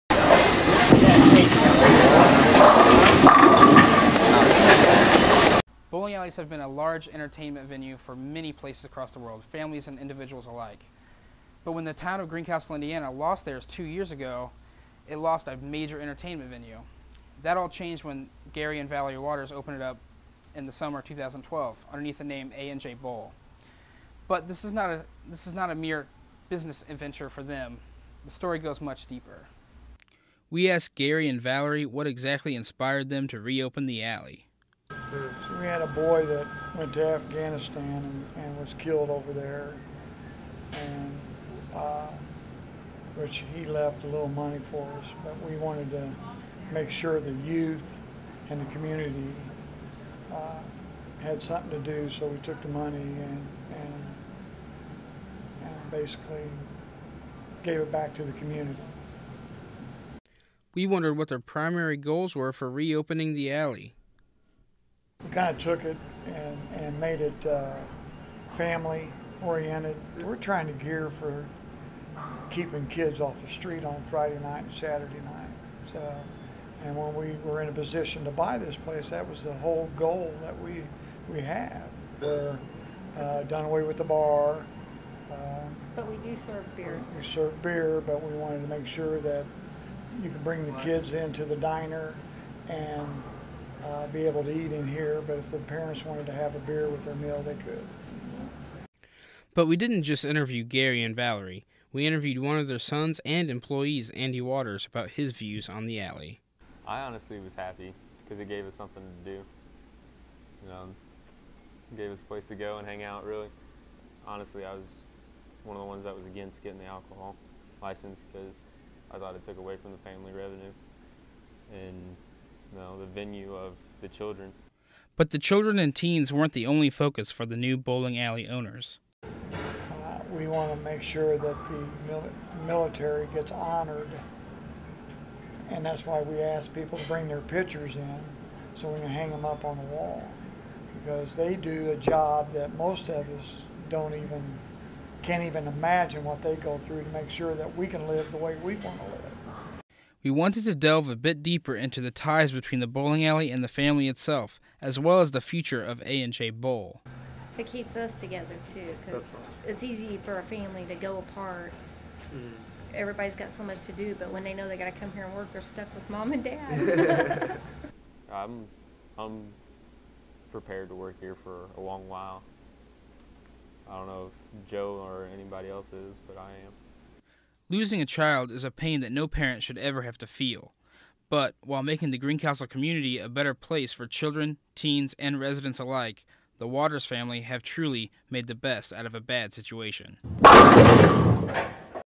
A-n'-J Bowl
This video is portraying the local bowling alley in Greencastle Indiana. The bowling alley closed around 2 years ago, and recently was re-opened by Garry and Valerie Waters of Cloverdale Indiana. The video shows what the Waters family has had to deal with in their lives in order to make this dream of opening the bowling alley a success.